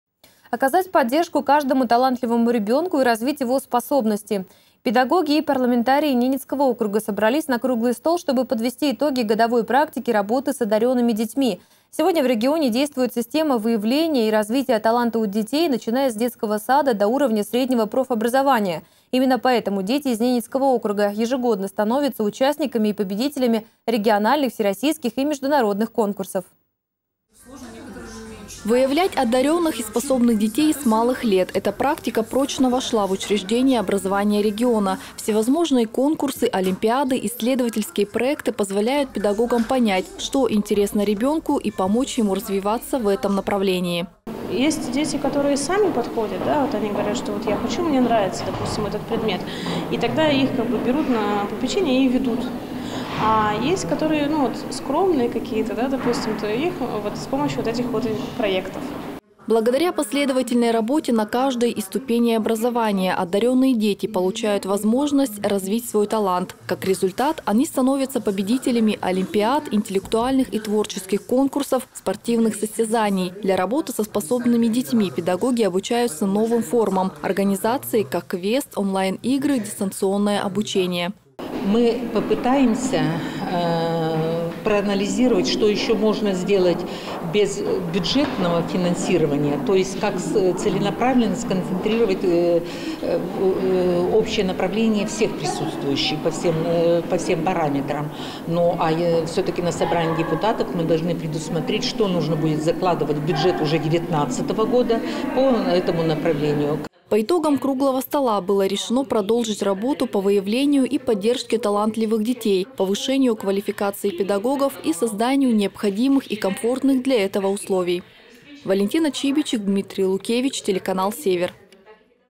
В НАО подведены итоги год работы с одаренными детьми
Оказать поддержку каждому талантливому ребёнку и развить его способности. Педагоги и парламентарии Ненецкого округа собрались на круглый стол, чтобы подвести итоги годовой практики работы с одарёнными детьми. Сегодня в регионе действует система выявления и развития таланта у детей, начиная с детского сада до уровня среднего профобразования. Именно поэтому дети из Ненецкого округа ежегодно становятся участниками и победителями региональных, всероссийских и международных конкурсов.